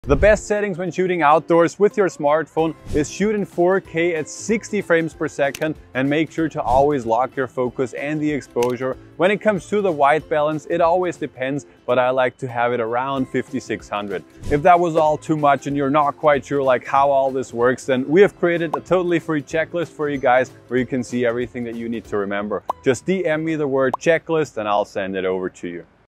Outdoor Videography Essentials: Mastering Smartphone Shooting
Improve your mobile video skills in just 5 Days with our starter course!

📱🌳 Master Outdoor Smartphone Shooting: Essential Tips Unveiled! 🌟🎬 In this Instagram Reel, I'm sharing easy-to-follow tips for creating stunning videos while embracing the beauty of the outdoors. 📸🏞️

Set the stage for cinematic brilliance with these three crucial settings: 🌠🔧
1️⃣ Shoot in 4K and 60 frames per second for breathtaking clarity and smoothness.
2️⃣ Lock the focus and exposure to maintain consistent visual excellence.
3️⃣ Set the white-balance to 5600 kelvin, allowing colors to pop in the natural light.

But wait, there's more! If you'd like a totally free checklist PDF to keep your outdoor shooting game on point, just DM me the word "Checklist," and I'll send it your way! 📝💌

Elevate your smartphone videography and immerse yourself in the wonders of nature. Share your outdoor adventures with cinematic flair and mesmerize your audience! 🌳🌞